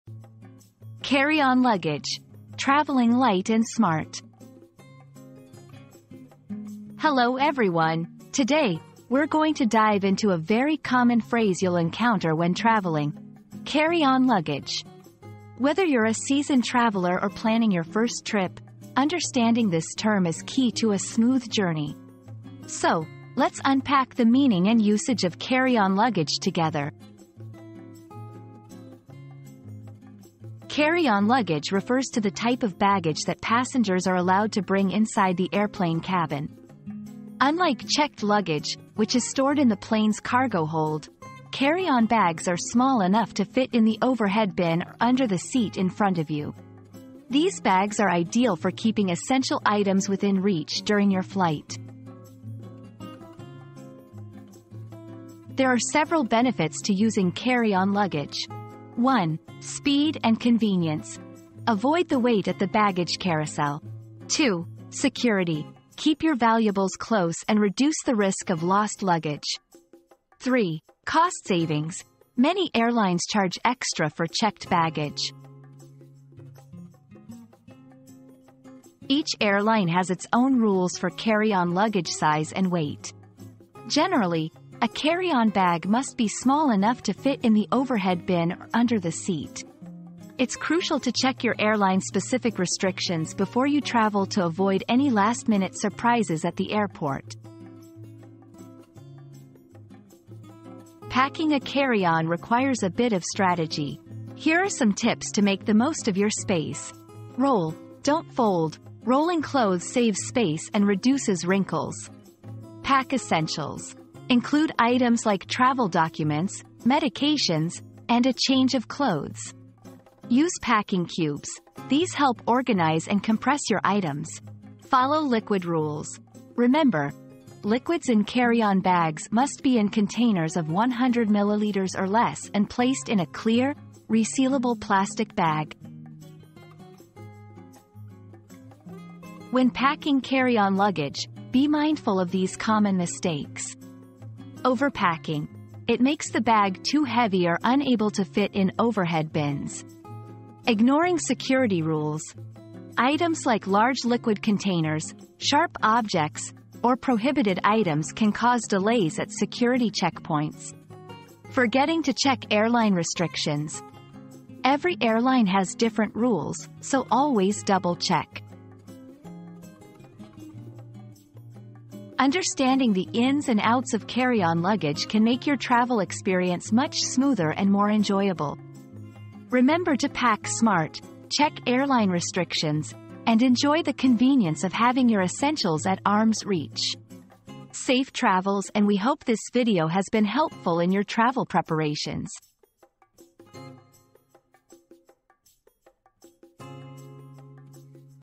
Carry-On Luggage: Traveling Light & Smart
Traveling Light & Smart: The Ultimate Guide to Carry-On Luggage • Discover the secrets to hassle-free travel with our ultimate guide to carry-on luggage. Learn how to pack efficiently, maximize space, and stay organized while jet-setting around the world. Say goodbye to heavy suitcases and hello to stress-free adventures!

00:00 • Introduction - Carry-On Luggage: Traveling Light & Smart
00:30 • What is Carry-On Luggage?
00:55 • Why Choose Carry-On Luggage?
01:18 • Size and Weight Restrictions
01:40 • Packing Tips for Carry-On Luggage
02:18 • Common Mistakes to Avoid